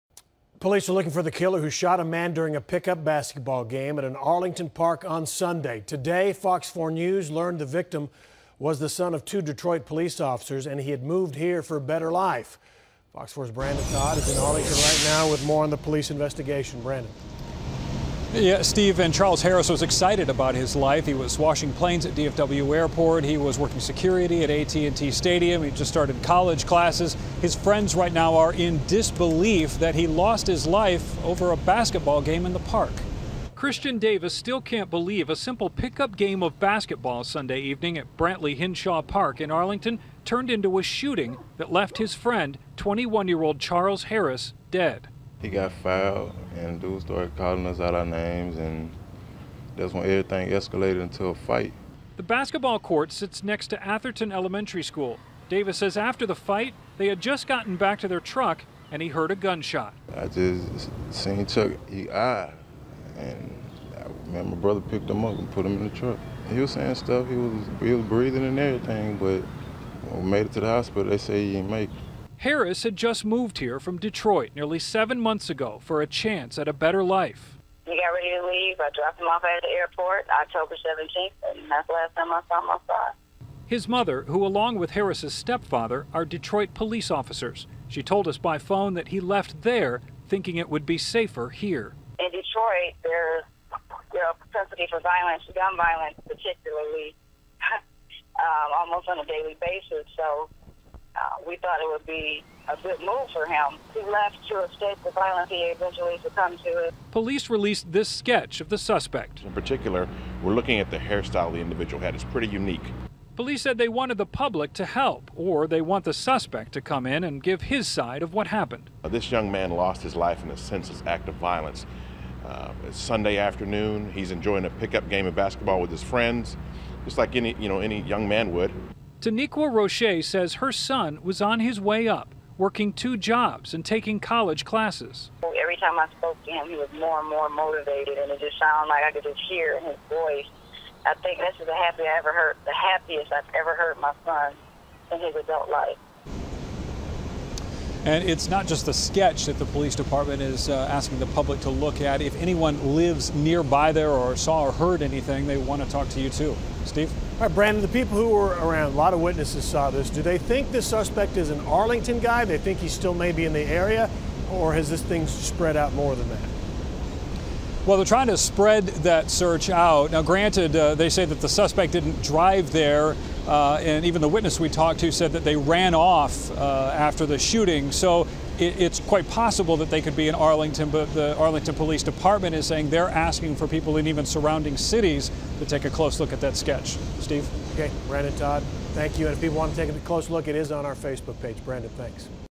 Arlington police seek suspect in deadly park shooting
Arlington police are asking for the public's help in identifying a suspect in connection to a deadly shooting that took place over the weekend.